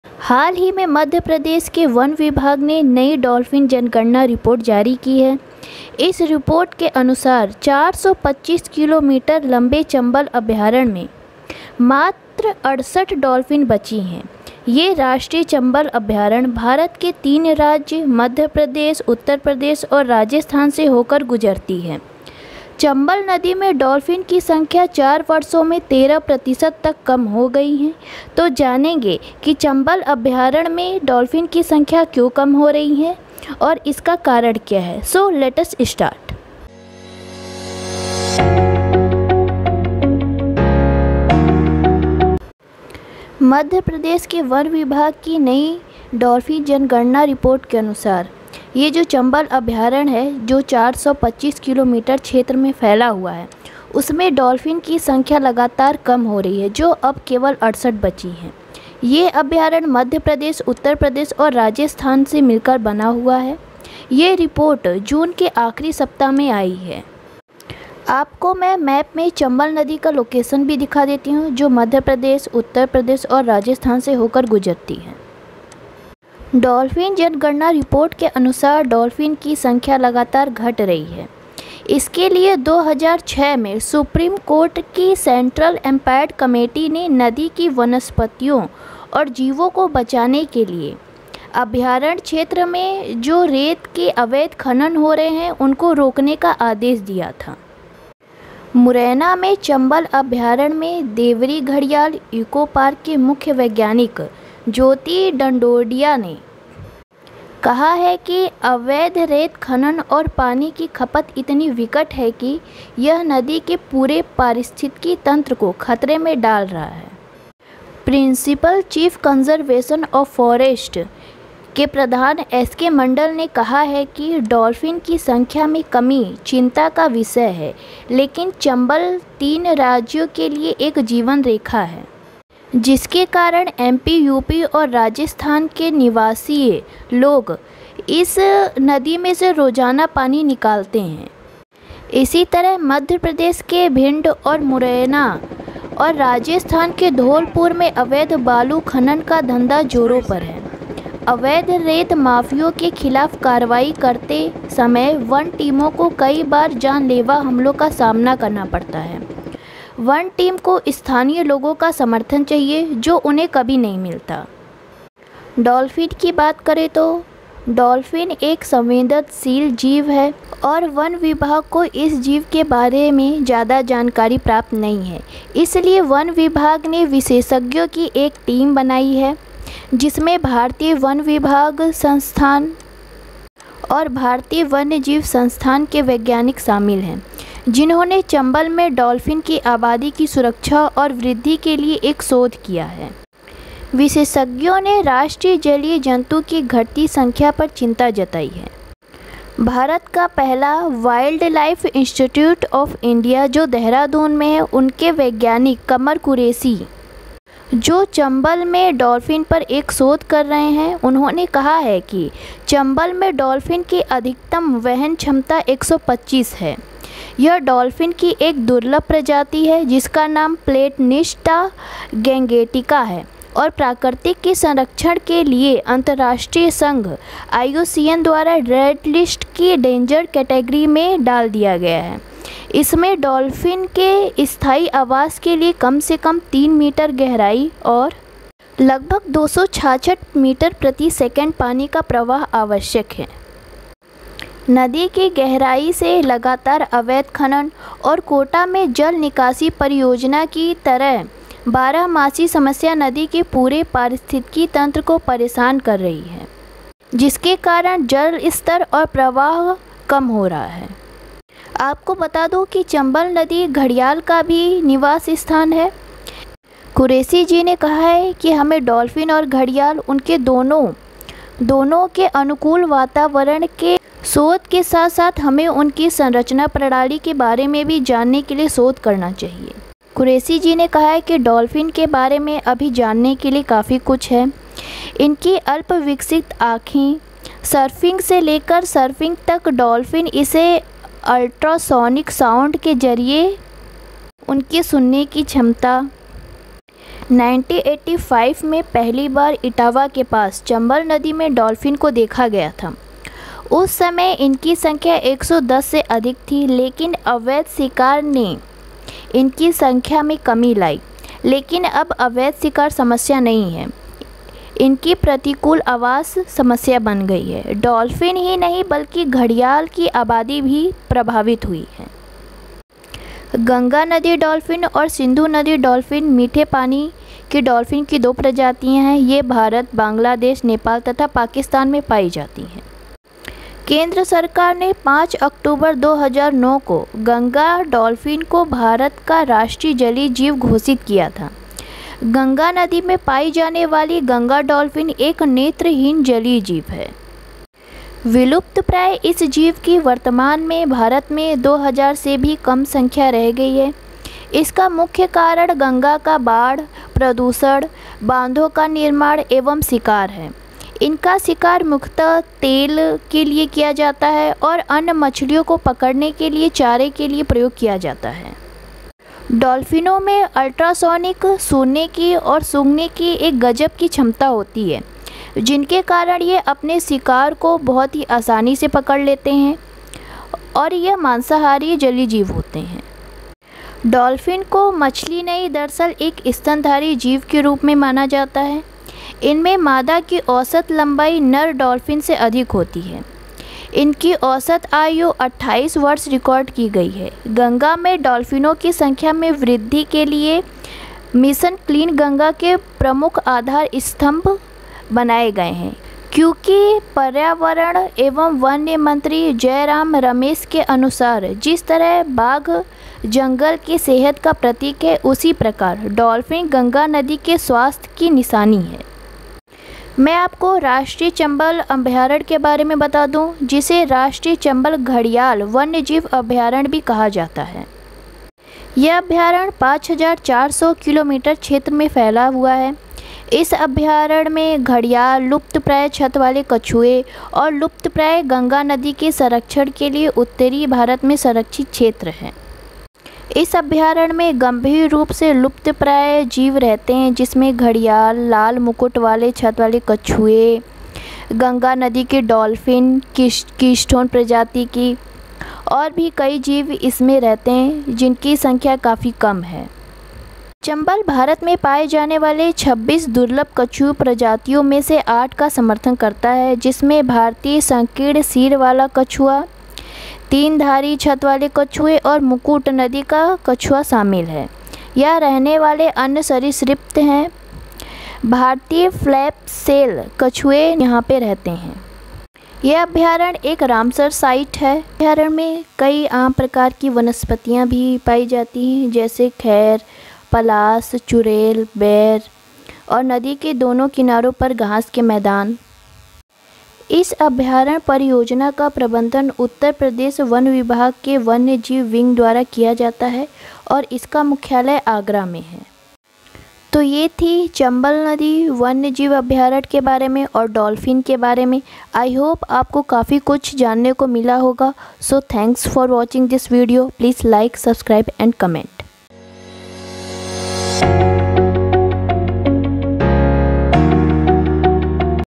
चंबल नदी में डॉल्फ़िन की संख्या कम हो गई है।The number of dolphins in the Chambal River has decrease
There are just 68 dolphins left in 425-kilometre-long Chambal river sanctuary which passes through three states (Madhya Pradesh, Uttar Pradesh and Rajasthan), according to the latest census report of Madhya Pradesh forest department. The report came out in last week of June.
According to the census report,  The decreasing trend is continuing from 2016 when there were 78 dolphins.
In 2006, the Supreme Court’s Central Empowered Committee (CEC) ordered a ban on mining in the sanctuary area to save the flora and fauna of the river. “But illegal sand mining and consumption of water is so rampant that it is putting the whole ecosystem of the river in danger,” said Jyoti Dandotiya, chief scientist of the Deori Gharial Eco-park in Chambal sanctuary in Morena.
Principal chief conservator of forest (PCCF), wildlife, SK Mandal said, “The decrease in number of dolphins is a matter of concern but Chambal is a lifeline for three states and the locals of MP, UP and Rajasthan are withdrawing water daily. Similarly, illegal sand mining is rampant in Bhind and Morena in MP and Dholpur in Rajasthan. Forest teams face so many life threatening attacks while taking actions against illegal sand mafia. Forest team needs support of locals, which they never get.”